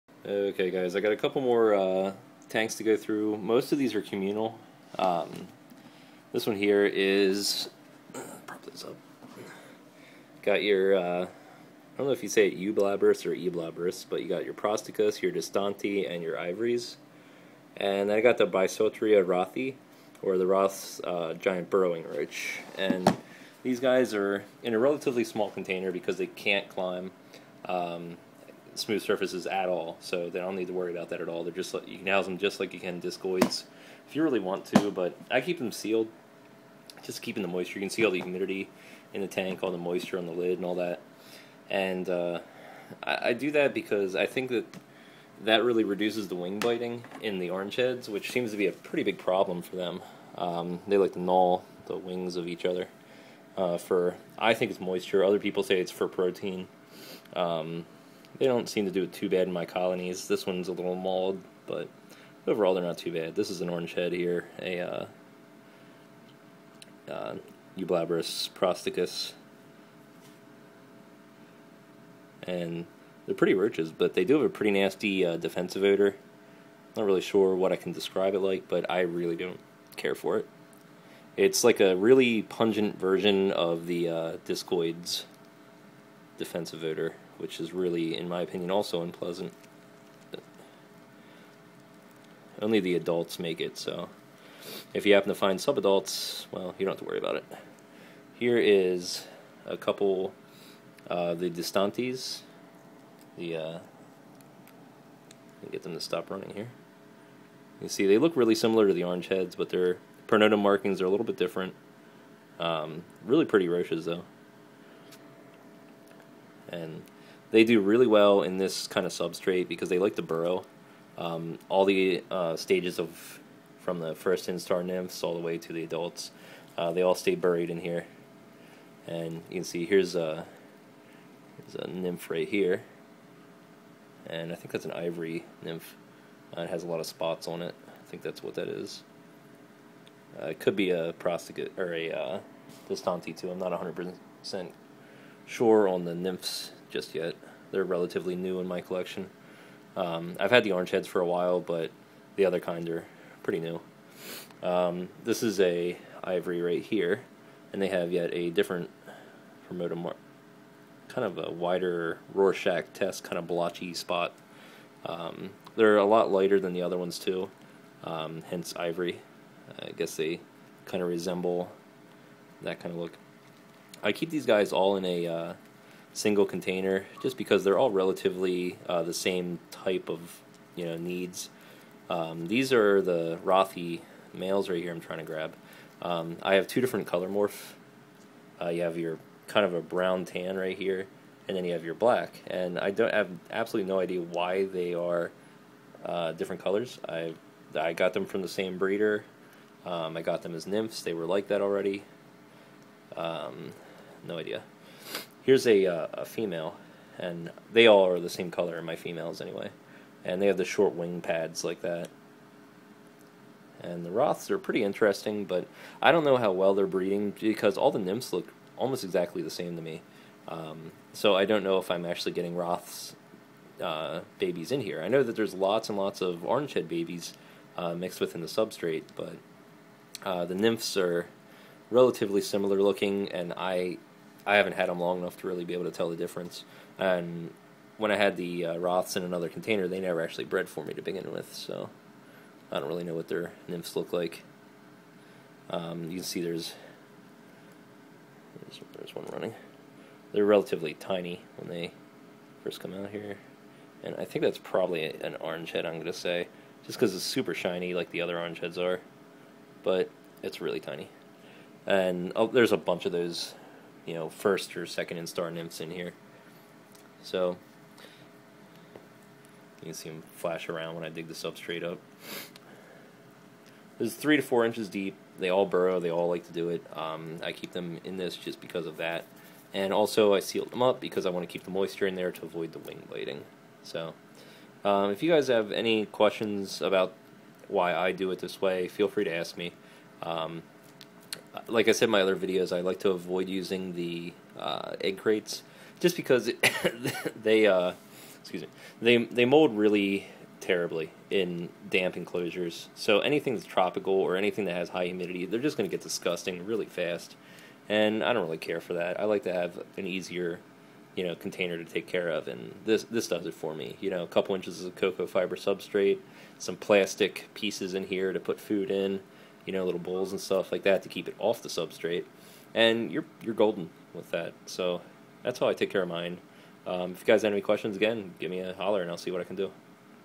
Eublaberus posticus orange head roach colony
This is my mixed species cockroach colony. I have Eublaberus prosticus which are the orange headed cockroach, Eublaberus distanti which I think are called 6 spotted cockroaches, Eublaberus "ivory" roa